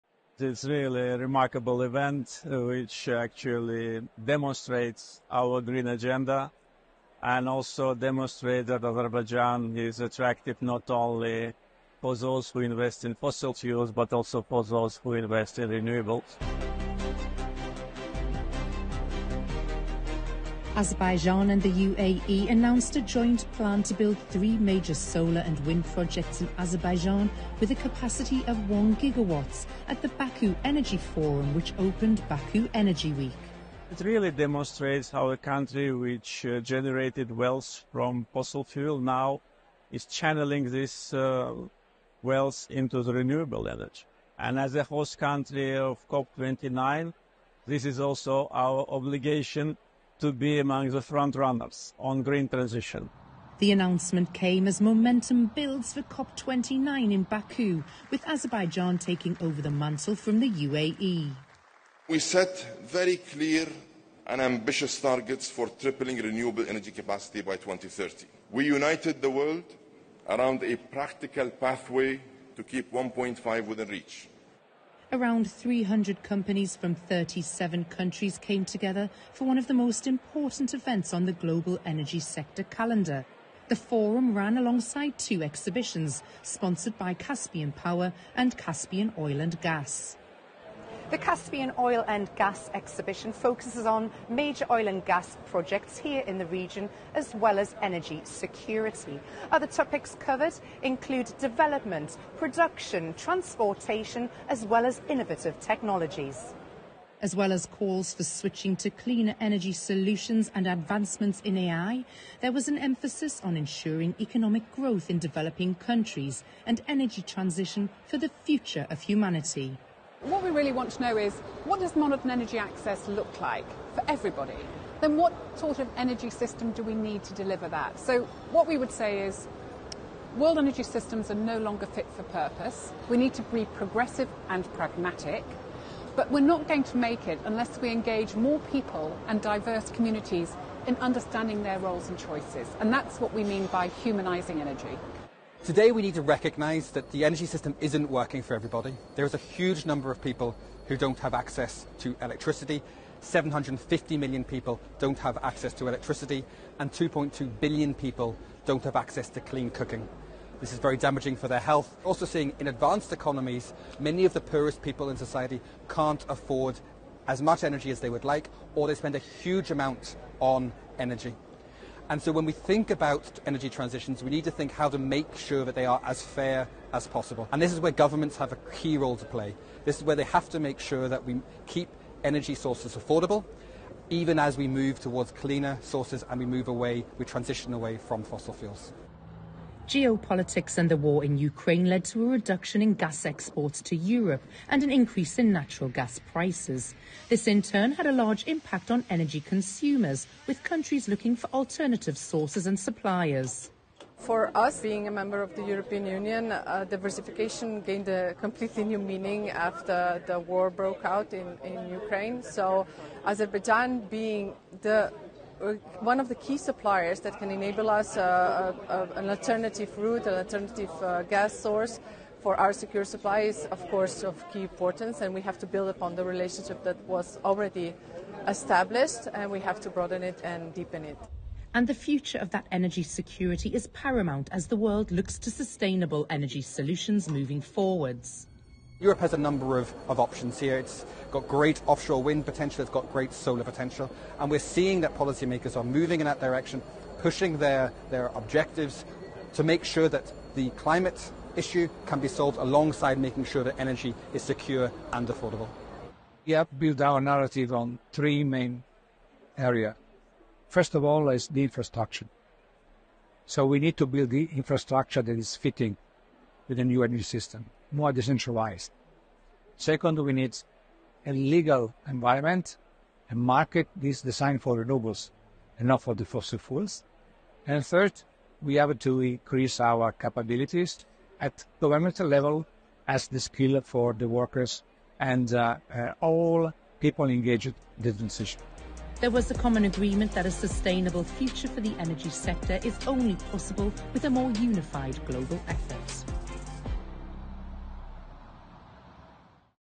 Euronews-un Bakı Enerji Forumundan reportajı.
21 iyun tarixində Euronews kanalında 29-cu Beynəlxalq Xəzər Neft və Qaz Sərgisi Caspian Oil&Gas və 12-ci Xəzər Beynəlxalq Enerji və Yaşıl Enerji Sərgisi Caspian Power haqqında reportaj yayımlandı. Bu sərgilər Bakı Enerji Həftəsi çərçivəsində Bakı Ekspo Mərkəzində keçirildi.

Eyni reportajda, Euronews ilə Azərbaycan Prezidenti İlham Əliyevin müsahibəsi də daxil edildi.